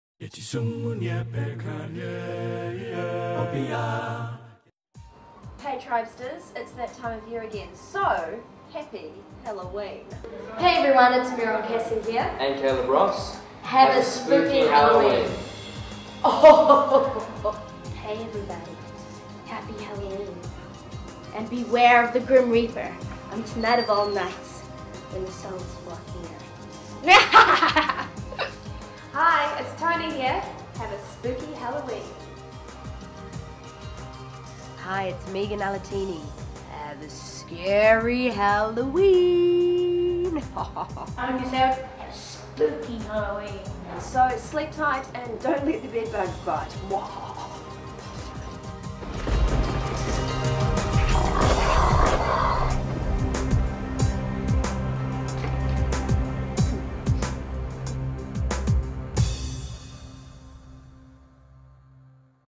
Happy Halloween from all The Tribe!
It's time for pumpkins, pumpkin pie, broomsticks, silly costumes - and a fun filled message from The Tribe cast for Halloween!